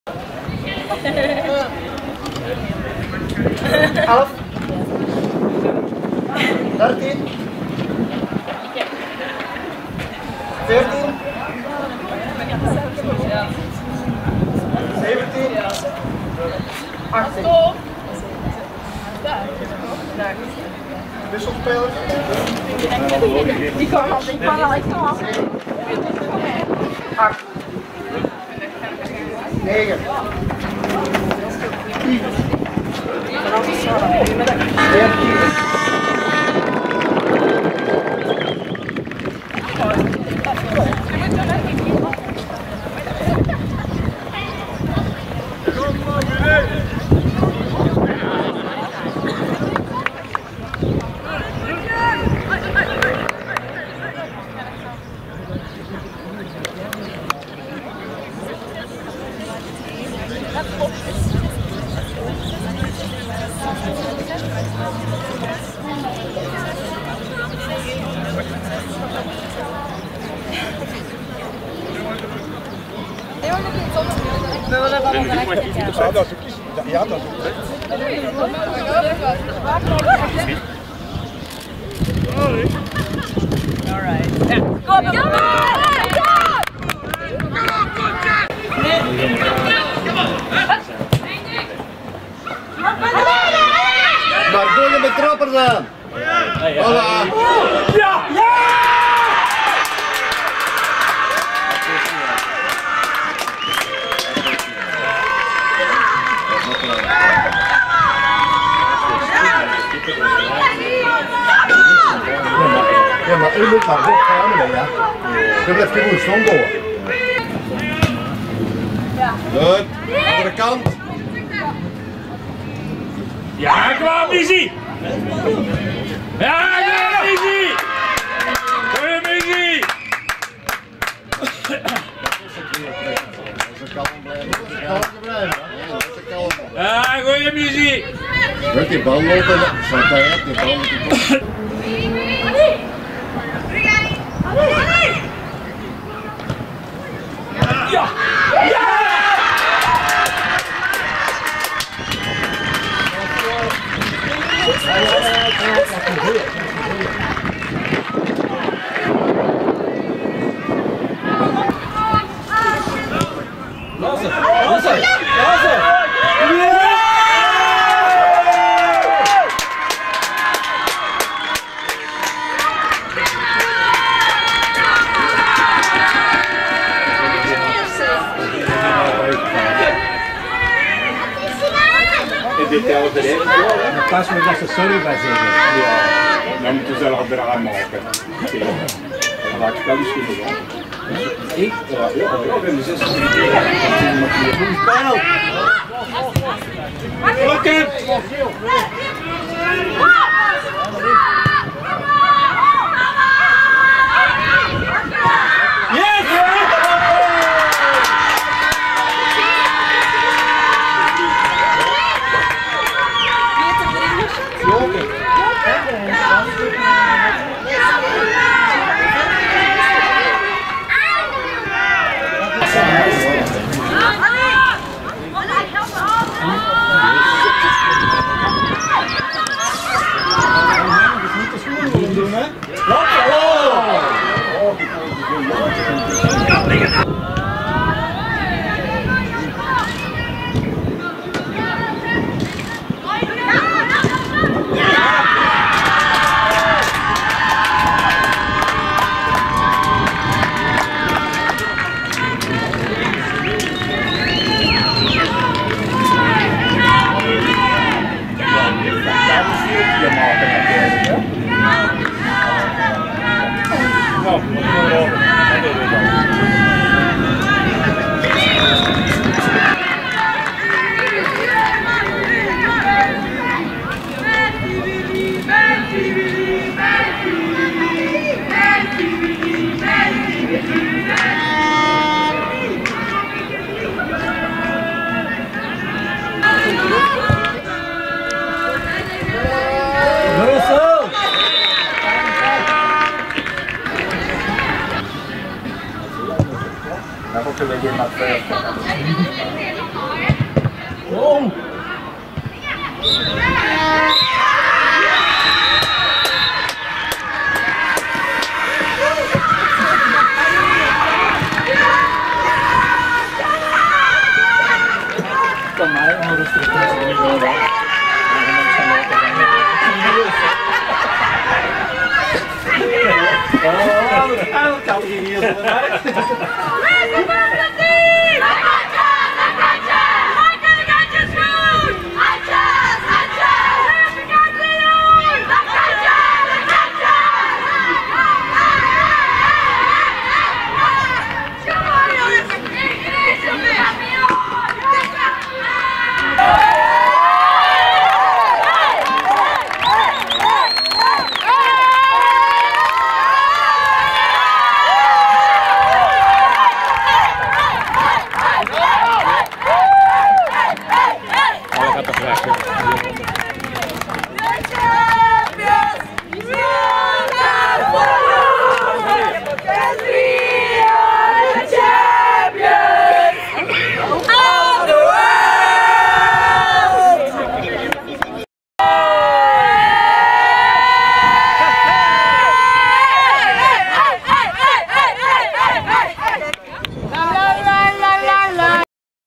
DVC Eva's Tienen - Kontich op 05.05.2018: de goals!
DVC Eva's Tienen - Kontich 1-5
De doelpunten :
14' Chiara Wielockx 0-1
21' Mizzy Raskin 1-1
42' Sarah Van Haute 1-2
50' Annelies Van Loock 1-3
52' Joke Verelst 1-4
69' Stefanie Van Broeck 1-5